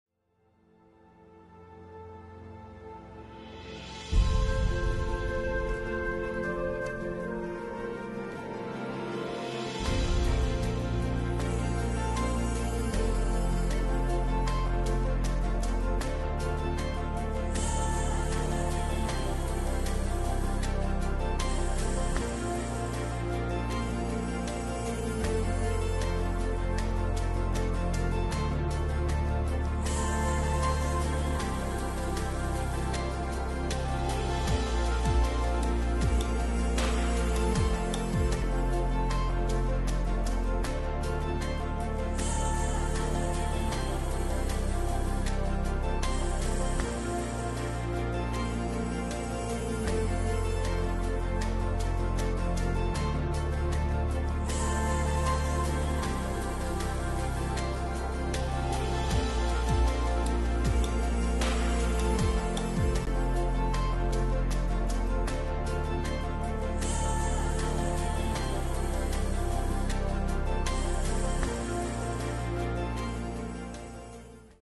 Eden Park. Cincinnati, Ohio
Drone footage of Eden Park in Cincinnati, Ohio.